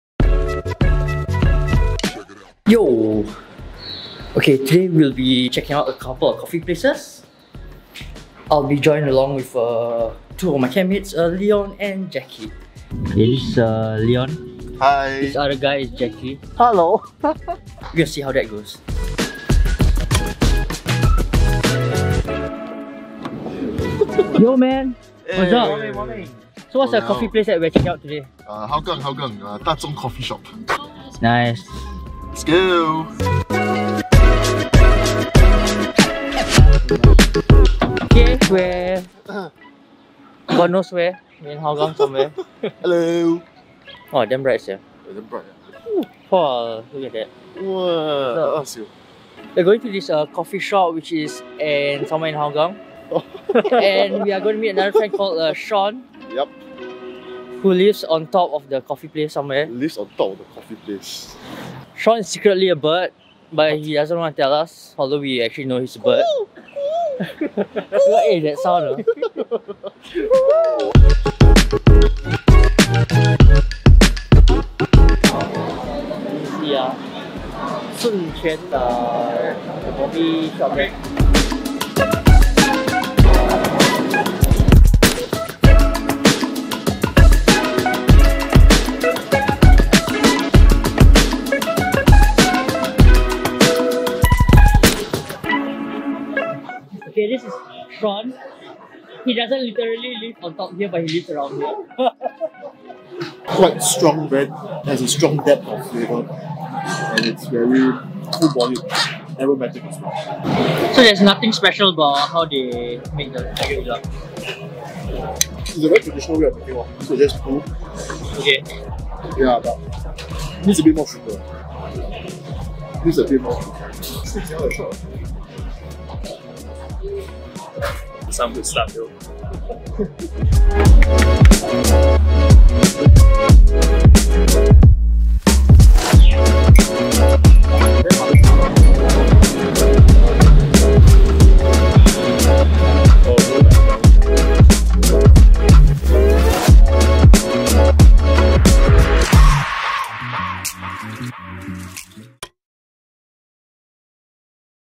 The BEST kopi (coffee) in Singapore? // Da Zhong Cafe // vlog with Canon m200 test
(2020) - With the lockdown almost over, we head out to explore coffee places and eateries around Singapore.

Is Da Zhong Cafe the best place to have your coffee?

Da Zhong Cafe
Blk 105 Hougang Ave 1 #02-20 Singapore 530105
Hougang Hainanese Village Centre
Operating hours 5am-2pm. Closed on Monday & Friday

Situated in Hainanese Food Village, Da Zhong Cafe serves what some consider to be the best cup of kopi in Singapore. With about 50 years of coffee-making experience under his belt, Uncle Tay is the man behind the delicious kopi that everyone has been raving about. Together with his wife, Uncle Tay runs the humble kopi shop focusing only on breakfast sets comprising toast, eggs, and coffee.

Just a random vlog to test out the Canon m200.